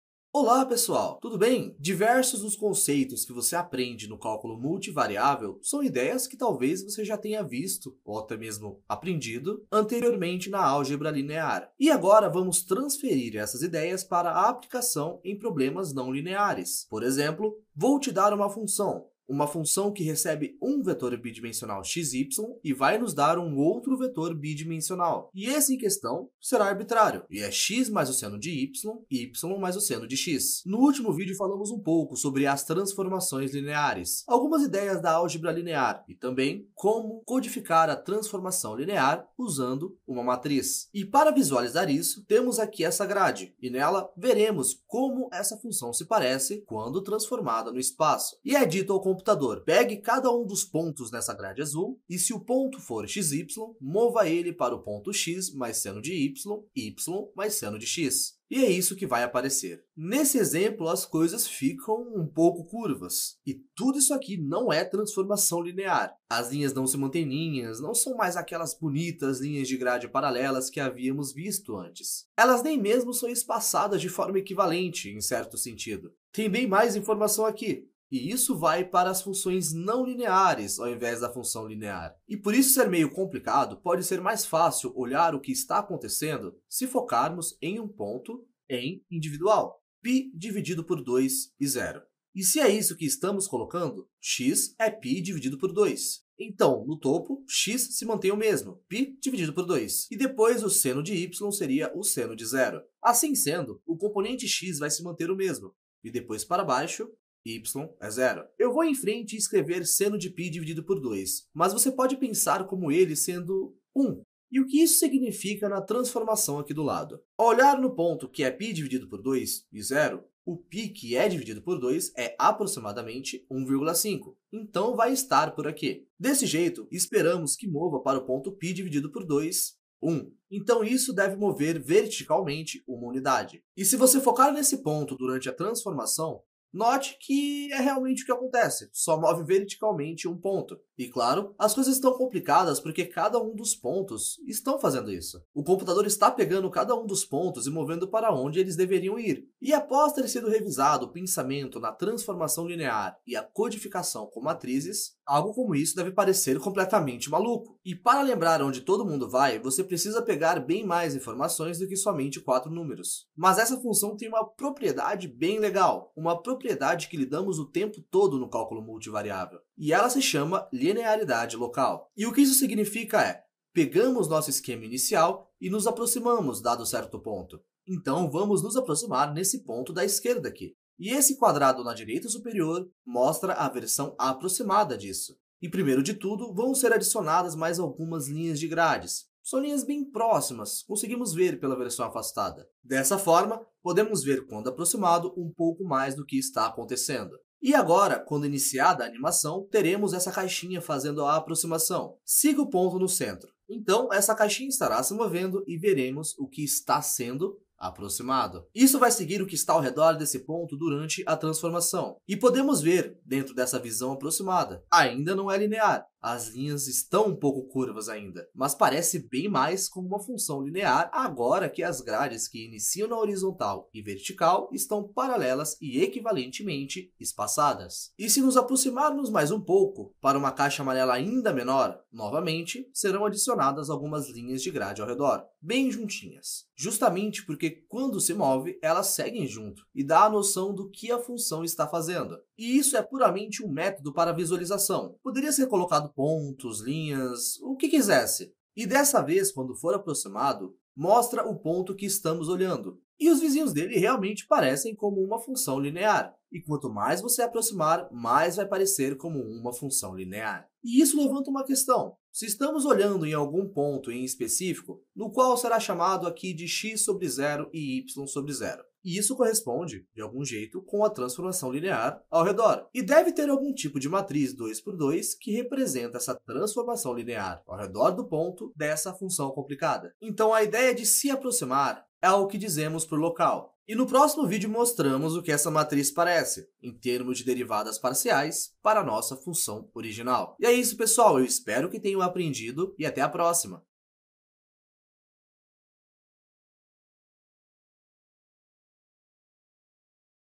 Linearidade local para uma função de múltiplas variáveis
Esse vídeo é uma representação visual da linearidade local para uma função com saída e entrada bidimensional. É um preparatório para aprender a Matriz Jacobiana.